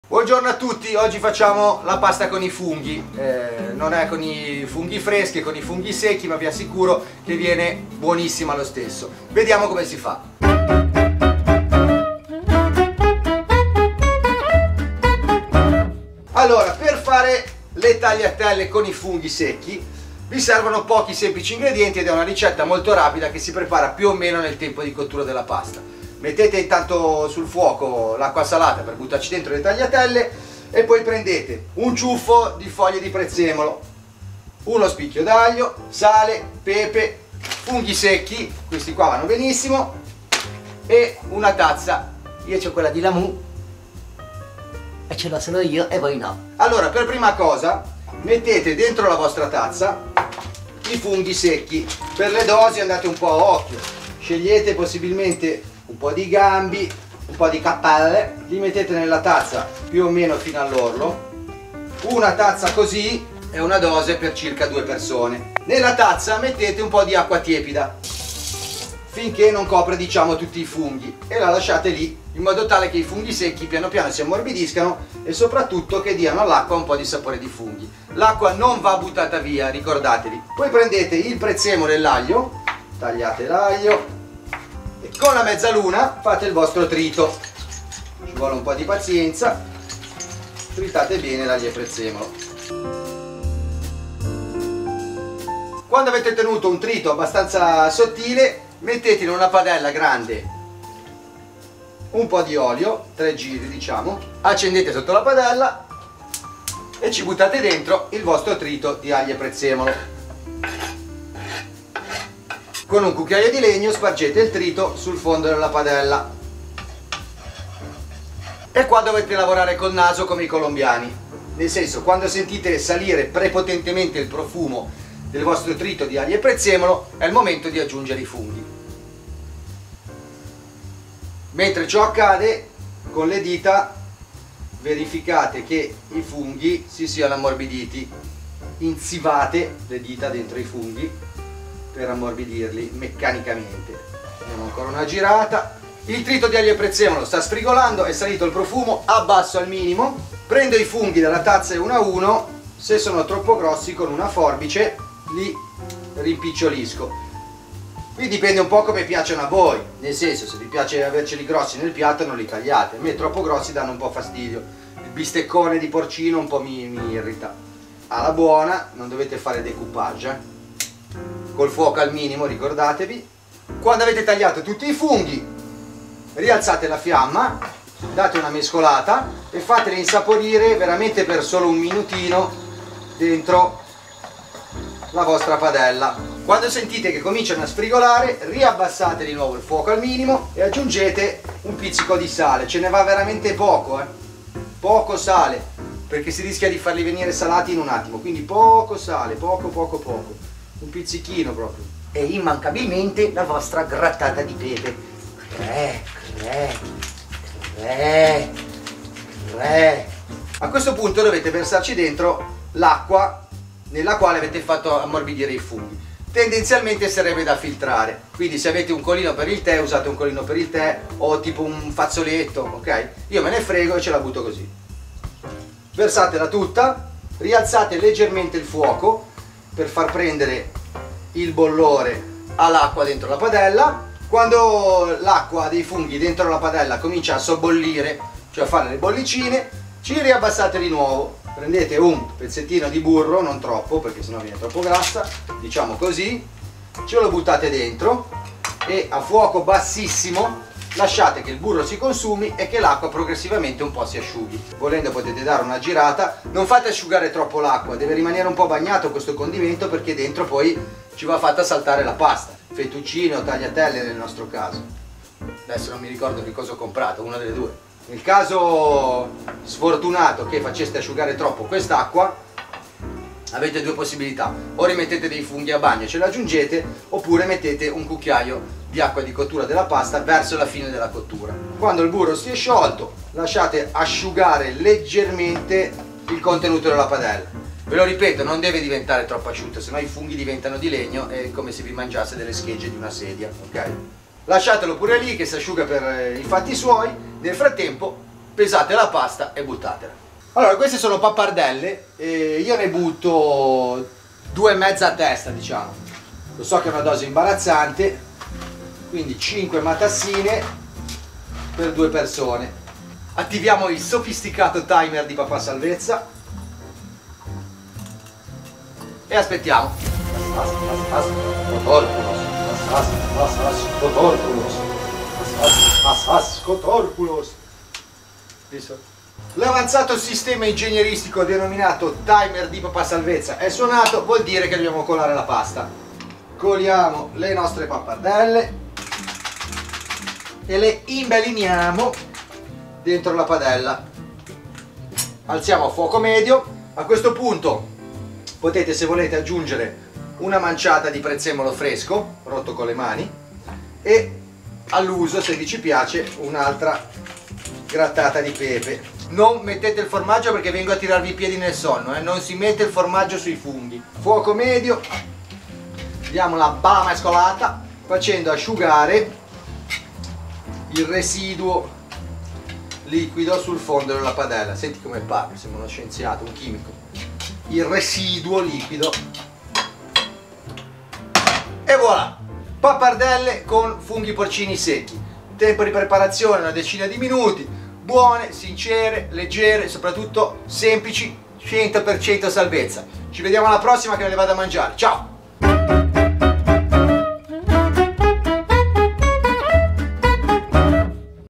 PAPPARDELLE COI FUNGHI
Facile , veloce e sempre buonissima.

INGREDIENTI:
- prezzemolo
- uno spicchio d'aglio
- funghi secchi
- una tazza di acqua tiepida
- olio sale e pepe nero
- burro

TEMPO DI PREPARAZIONE: 15 min.
TEMPO DI COTTURA: 10 min.
GRADO DI SALVEZZA: 100%

Le pentole con cui mi trovo bene
Le padelle che uso le trovi qui